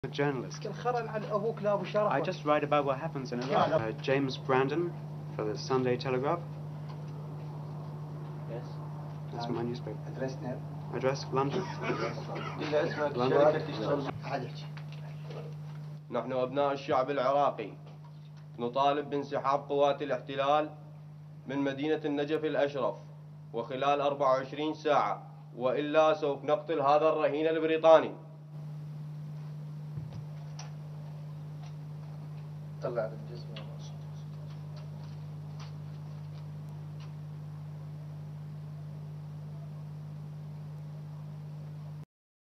British journalist taken hostage
APTN, IN COMMON WITH OTHER AGENCIES, HAS BEEN GIVEN ACCESS TO THIS FOOTAGE SHOT BY A LOCAL CAMERAMAN WHO WAS TAKEN TO WHERE JAMES BRANDON IS BELIEVED TO BE HELD

1. Close up of kidnapper putting bandage on hostage James Brandon's head
2. UPSOUND: (English) James Brandon, British hostage, talking to kidnappers as he is bandaged:
"I am a journalist, I just write about what happens in Iraq."
3. SOUNDBITE: (English) James Brandon, British hostage:
"James Brandon for the Sunday Telegraph, that's my newspaper.
(kidnapper asks: "address?")
4. SOUNDBITE (English) James Brandon, British hostage:
"London"
5. SOUNDBITE (Arabic) Masked kidnapper as James Briton stands beside him:
"We are the people of Iraq demanding the occupation troops withdraw from Najaf in 24 hours. Otherwise we will kill this British hostage."
6. Brandon looking straight to camera

STORYLINE

Militants in the southern city of Basra stormed into a hotel, kidnapped a British journalist and threatened to kill him if U.S. forces did not leave the holy city of Najaf within 24 hours, according to police and a video released Friday.

In the video, the journalist, barechested and with a bandage on his head, identified himself as James Brandon, 23, a reporter for The Sunday Telegraph.

The "Telegraph, that's my paper," he said, turning to a masked captor. Asked to give his address, the clearly terrified man said: "London."

The militants said they had taken Brandon hostage in protest of the U.S. military presence in Najaf, one of the holiest cities for Shiite Muslims.

The kidnappers took a freelance cameraman to an undisclosed location on Friday morning to film their British hostage.

The U.S. military and Iraqi forces began a major offensive in the city Thursday designed to rout a rebel militia they had been battling there for more than a week.

"We are the sons of the Iraqi people," said the captor wearing a black ski mask. "We demand the withdrawal of the occupation forces from the holy city of Najaf in 24 hours, otherwise we will kill this British hostage," he said, patting Brandon on the shoulder.

Brandon was kidnapped on Thursday when about 30 gunmen, some dressed as police, stormed into the Diafa Hotel in Basra, said police Capt. Hashem Abdullah.

Witnesses later told APTN on Friday that two Iraqis dressed as policemen kidnapped Brandon from his room around 11.15pm Thursday night (19:15 GMT).

They said the two men shot Brandon twice in the legs before dragging him down hotel stairs and forcing him into a Toyota pick up truck.

Witnesses said the kidnappers were met outside the hotel by six Iraqis dressed in civilian clothes, who were driving a KIA minibus.

Hotel owner Mohammed Uglah said gunmen found Brandon and shot at him after he tried to escape, beating him over the head before taking him away. He'd earlier said that the journalist was shot twice in the leg.

Hotel staff showed a check-in form filled in by the man, identifying himself as James Brandon, 23. According to the form, he had arrived in Basra on Wednesday.

In Britain, the Sunday Telegraph confirmed Brandon had written stories for the paper.

"James Brandon was in Basra filing material for this Sunday's newspaper amongst other projects," Sunday Telegraph Deputy Editor Matthew d'Ancona said.

He could not confirm that Brandon had been abducted but said: "we are pursuing his situation with the greatest concern."

Brandon was the third journalist kidnapped in Iraq in recent months.